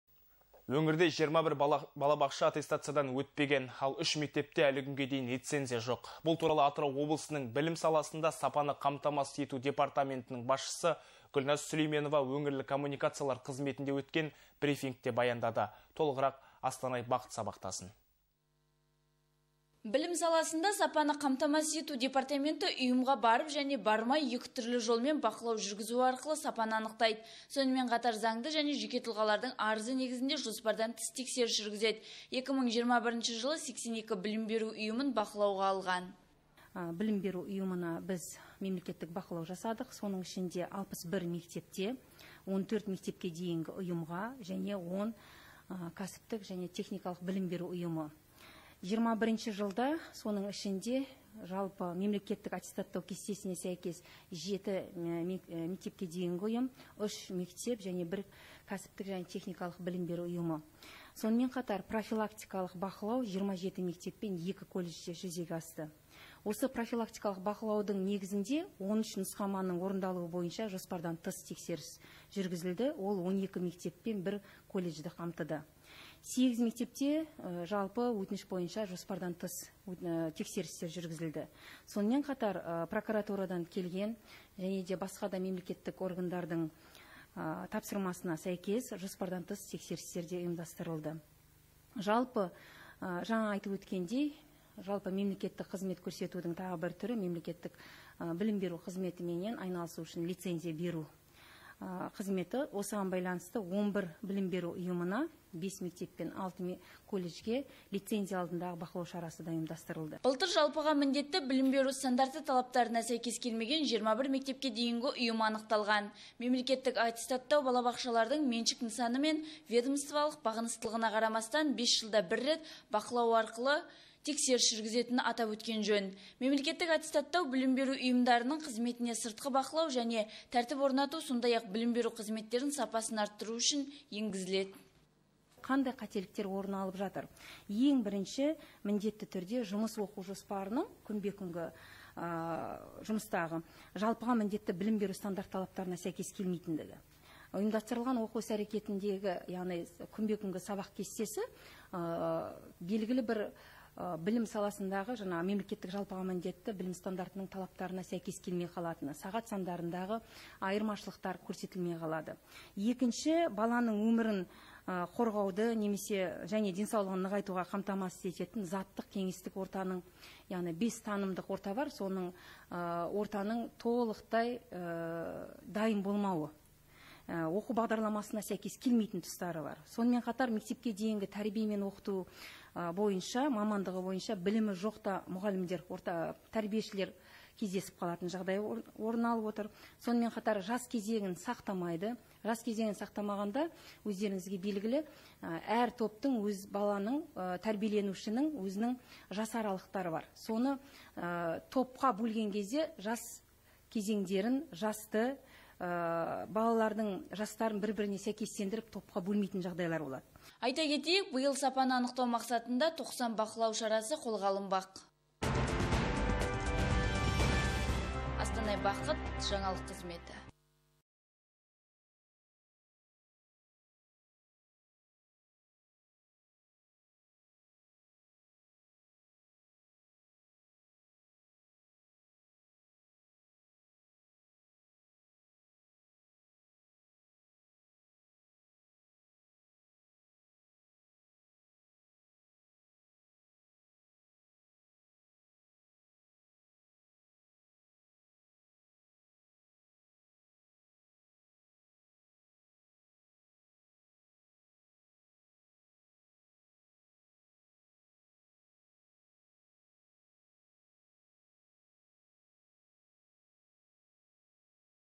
Өңірде 21 балабақша аттестациядан өтпеген
Өңірде 21 балабақша аттестациядан өтпеген. Ал, 3 мектепте әлі күнге дейін  лицензия жоқ . Бұл туралы  Атырау облысының білім саласында сапаны қамтамасыз ету департаментінің басшысы Гүлназ Сүлейменова  өңірлік  коммуникациялар қызметінде өткен  брифингте  баяндады. Толығырақ Астанай Бақыт сабақтасын